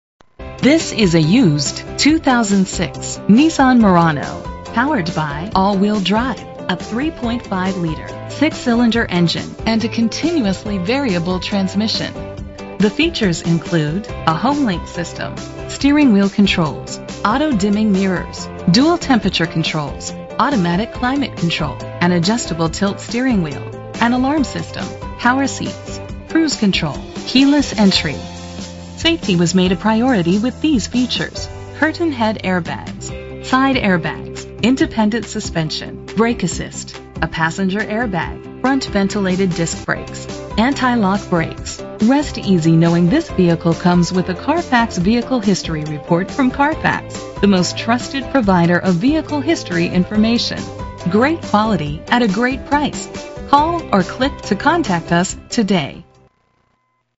2006 Nissan Murano - Centennial CO
This vehicle represents similar vehicles available at Arapahoe Hyundai.
Year: 2006
Make: Nissan
Model: Murano
Trim: SL
Engine: 3.5 liter 6 cylinder 24 valve MPFI DOHC
Transmission: Automatic CVT
Color: Glacier Pearl
Mileage: 124424
Address:
9899 W. Arapahoe